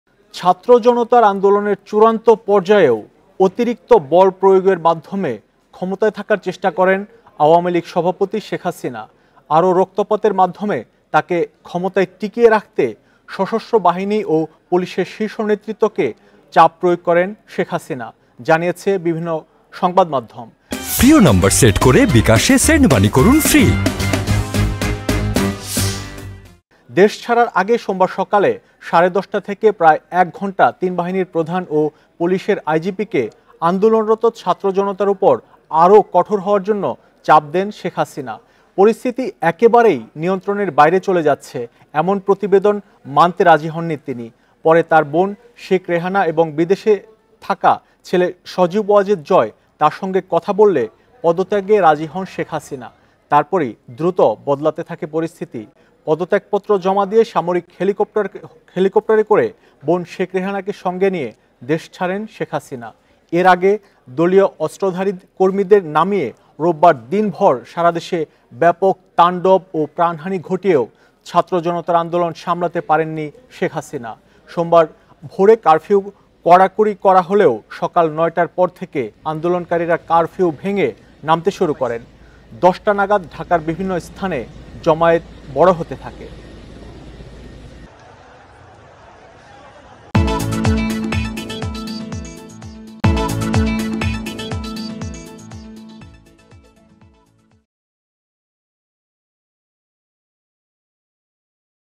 শেষ মুহূর্তে ৩ বাহিনীর প্রধান ও পুলিশের আইজিপিকে যা বলেছিলেন শেখ হাসিনা | Independent TV
ছাত্র-জনতার আন্দোলনের চূড়ান্ত পর্যায়েও অতিরিক্ত বল প্রয়োগের মাধ্যমে ক্ষমতায় থাকার চেষ্টা করেন আওয়ামী লীগ সভাপতি শেখ হাসিনা। আরও রক্তপাতের মাধ্যমে তাকে ক্ষমতায় টিকিয়ে রাখতে সশস্ত্র বাহিনী ও পুলিশের শীর্ষ নেতৃত্বকে চাপ প্রয়োগ করেন শেখ হাসিনা, জানিয়েছে বিভিন্ন সংবাদ মাধ্যম।
Awami League president Sheikh Hasina tried to stay in power by using extra force even at the final stage of the student-public movement. Sheikh Hasina exerted pressure on the top leadership of the armed forces and police to keep her in power through more bloodshed, various media reported.